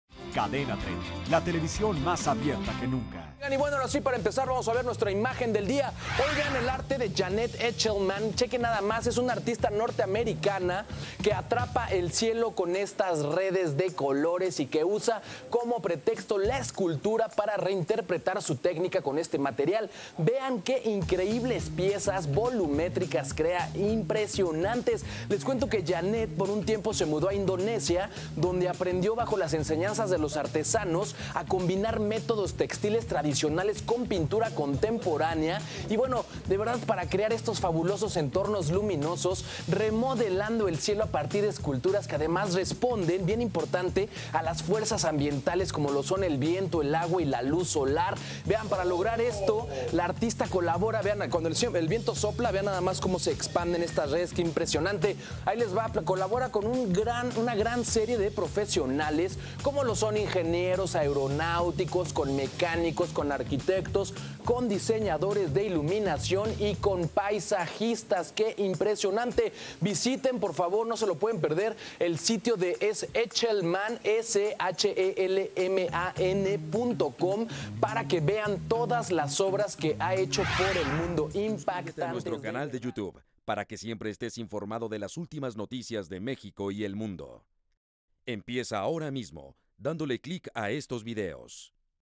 Redes de colores vuelan las ciudades de todo el mundo /las etéreas vorágines de Janet Echelman
Inspirada en las redes de los pescadores de un pequeño pueblo indio, la artista Janet Echelman ideó impresionantes figuras volátiles que, luminosas, se han cernido sobre importantes cielos urbanos.